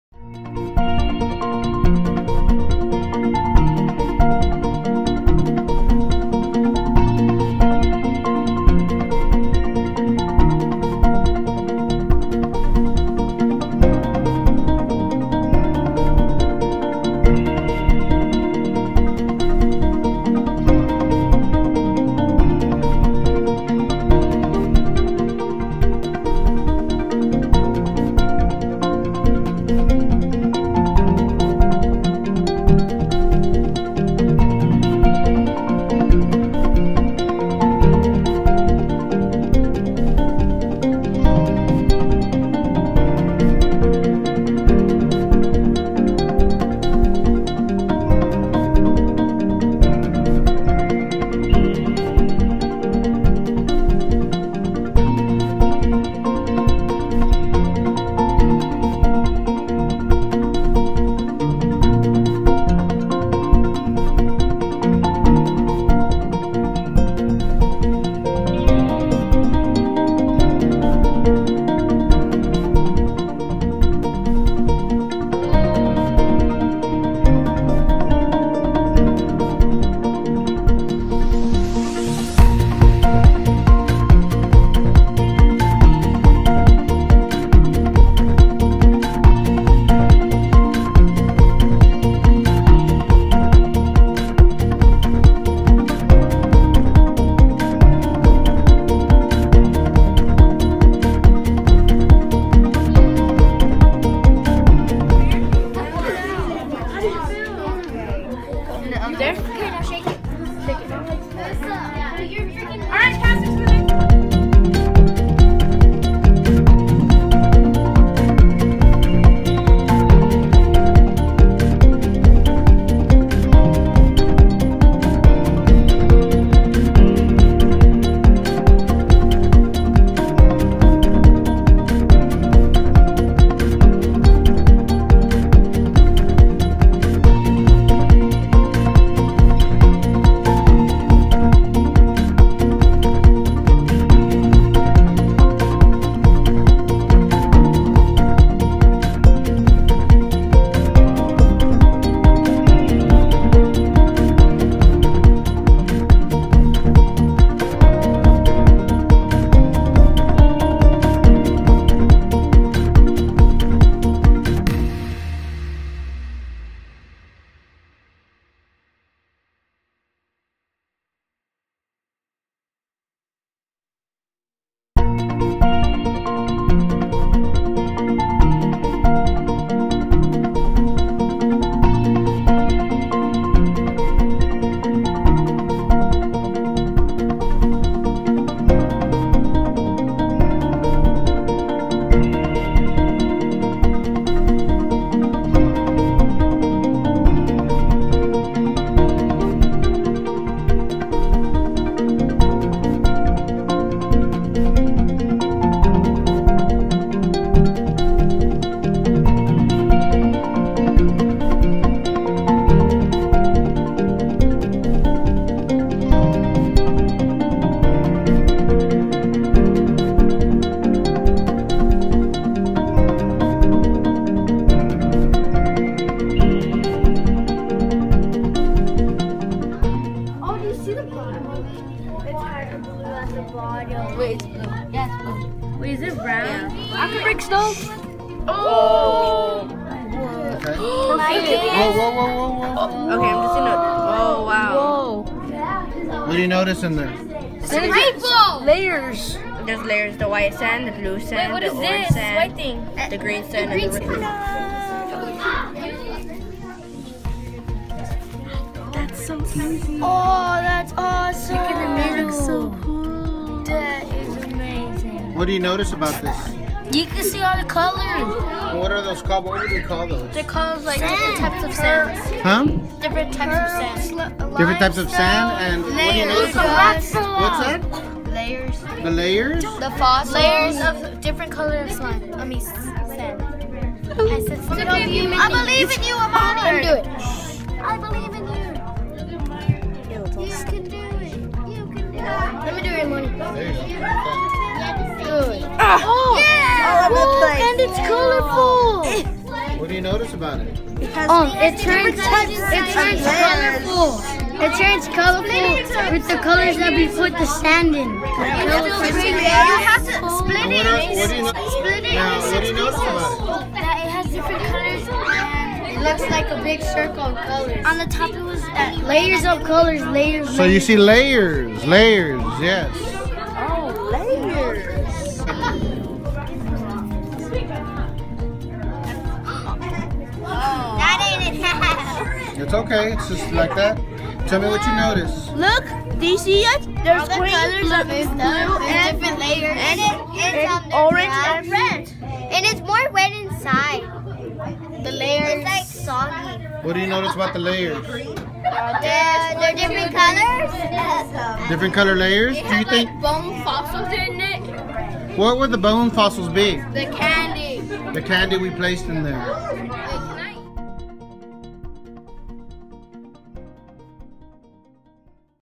Sedimentary Rock Lab
Cable ES 5th Grade Students work as geologists to learn how sedimentary rocks are formed through weathering, erosion, deposition, compacting, and cementation. NISD, San Antonio